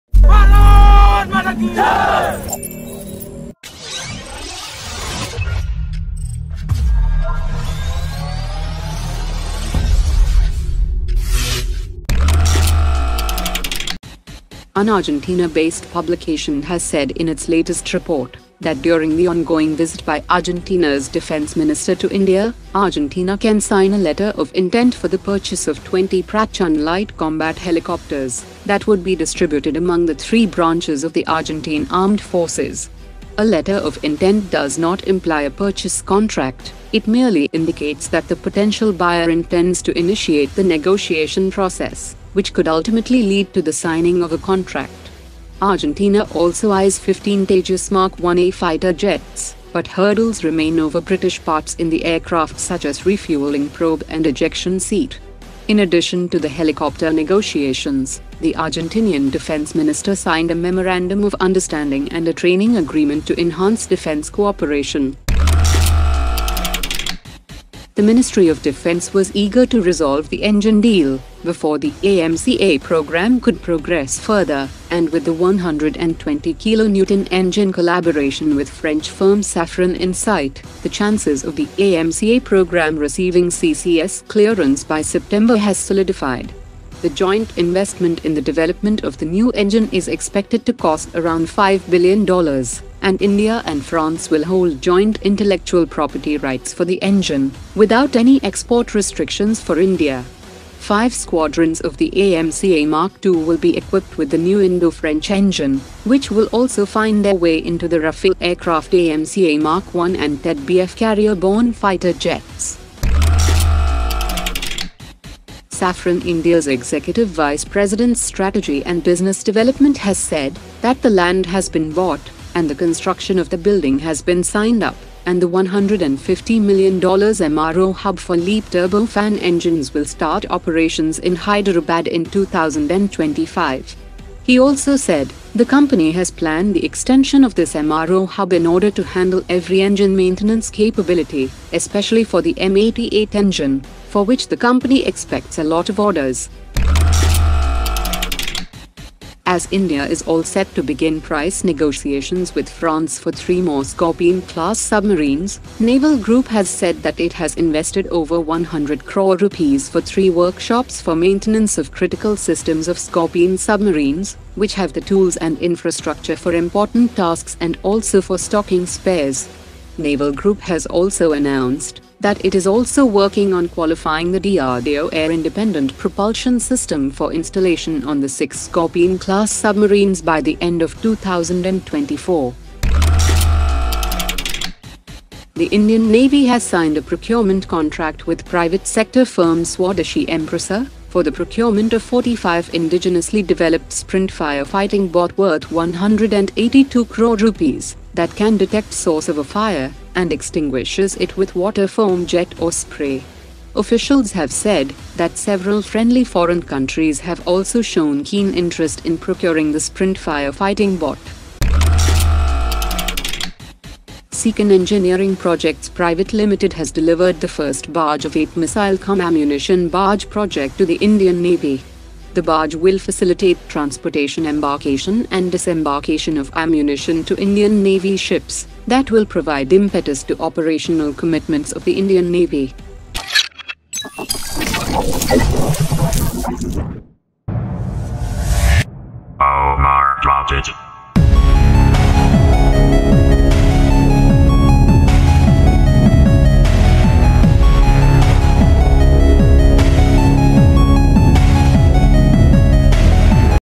Indian Defence Updates : Rafale Engine Hub,Argentina 20 Prachand Order,Indo French Engine For AMCA
1. Argentina aims to sign a letter of intent for 20 HAL Prachand helicopters
2. AMCA program set to soar with India France engine collaboration
3. Safran MRO Hub in Hyderabad extension to include M-88 Engines
4. Naval Group working on qualifying DRDO AIP on Scorpenes
5. Swadeshi Empresa wins 182 cr order for Sprint fire fighting bot order from Indian Navy
6. First barge of series LSAM7 delivered to Indian Navy